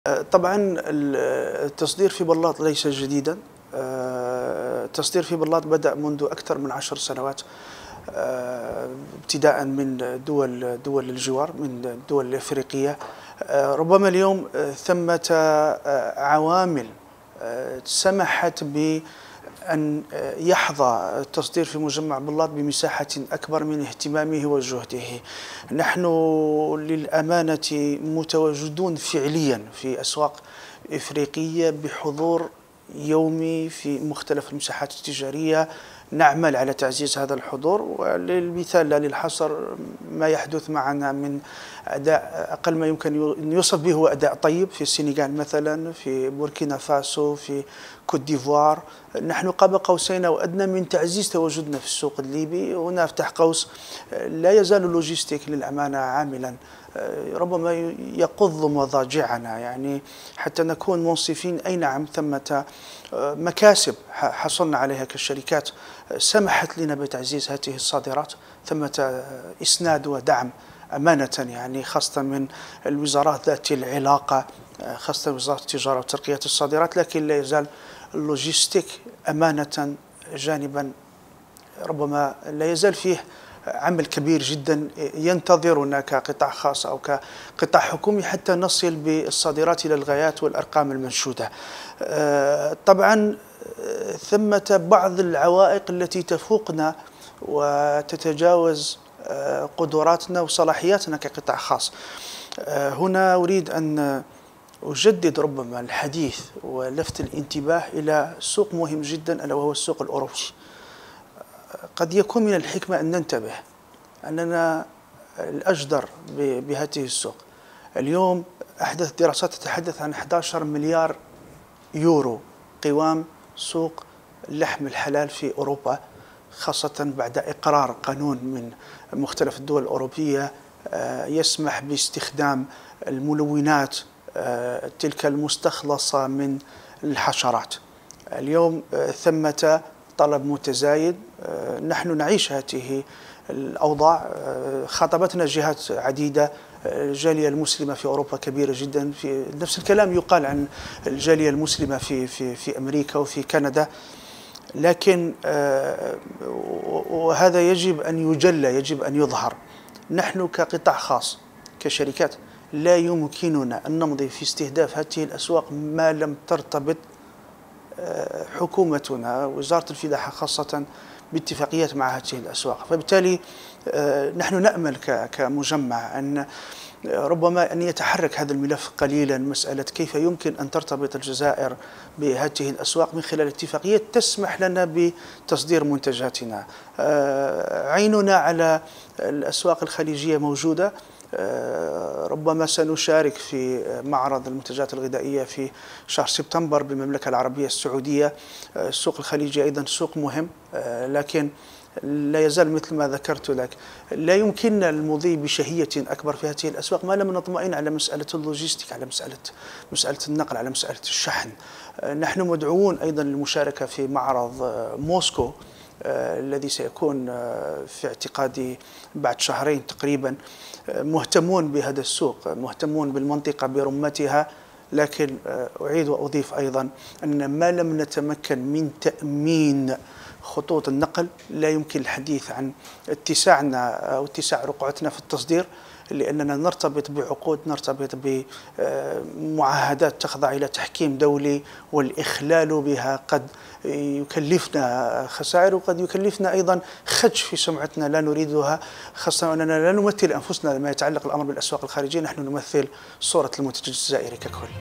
ملاح.." نعتمد على خطة واسترتيجية للتسويق خارج الجزائر كما نبرز تواجدنا بالمساحات الكبيرة دوليا"
المدير العام لمجمع بلاط مراد_ملاح.." نعتمد على خطة واسترتيجية للتسويق خارج الجزائر كما نبرز تواجدنا بالمساحات الكبيرة دوليا"